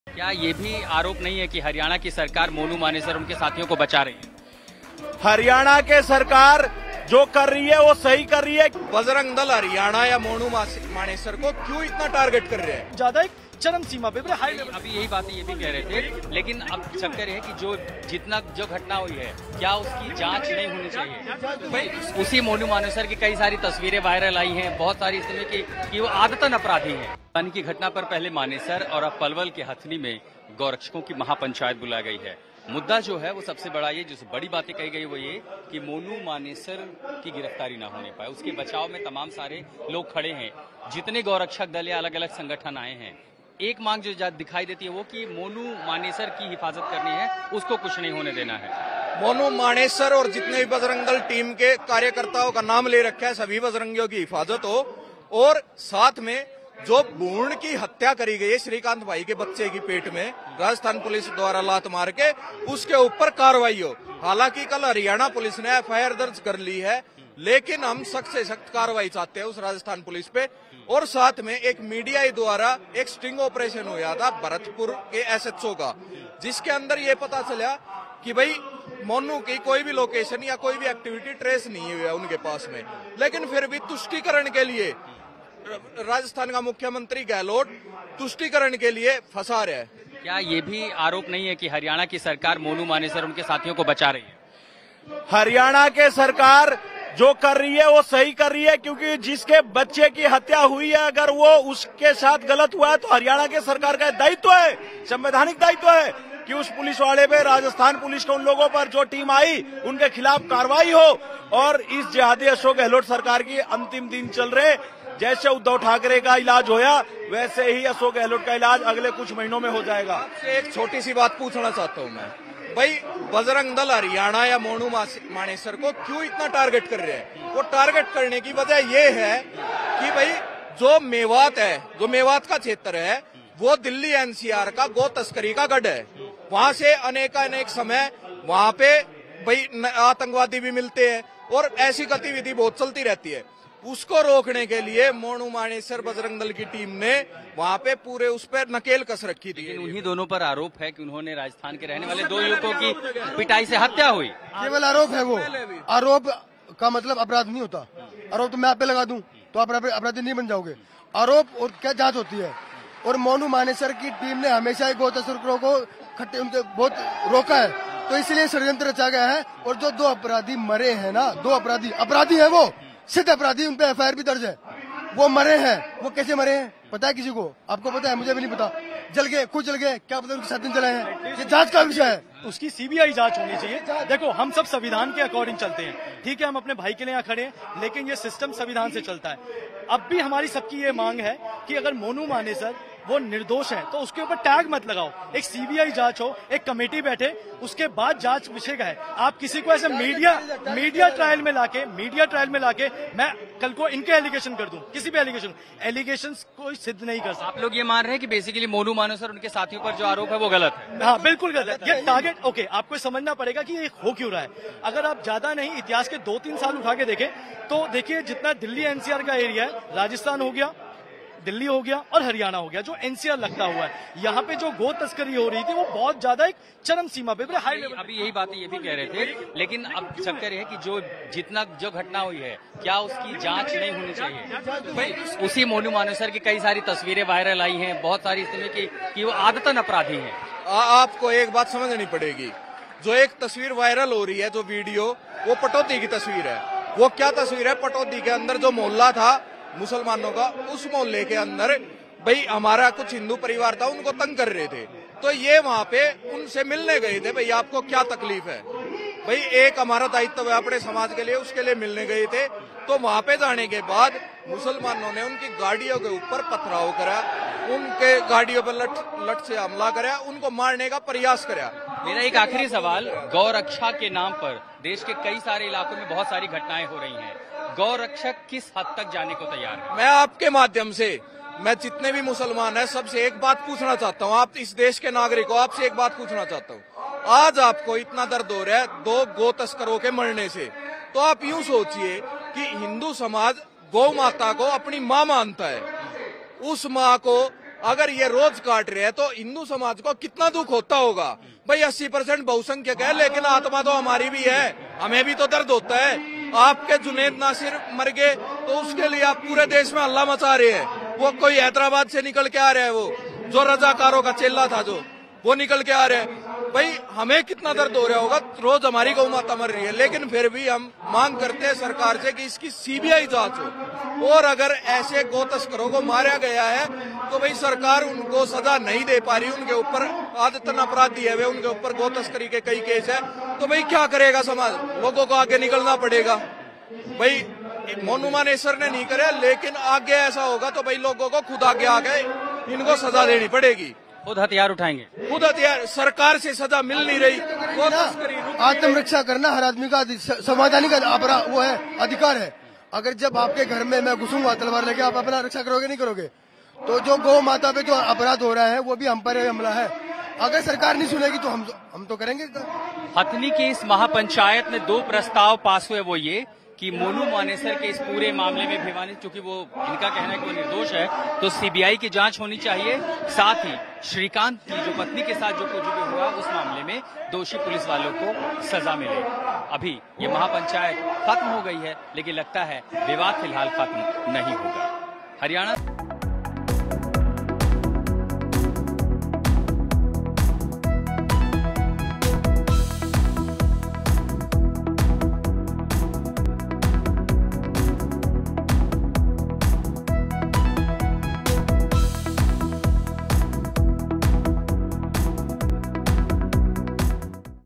मोनू मानेसर को लेकर हथिनी की महापंचायत में पास किया गया ये प्रस्ताव | Haryana Tak
मोनू मानेसर को लेकर हथिनी की महापंचायत में पास किया गया ये प्रस्ताव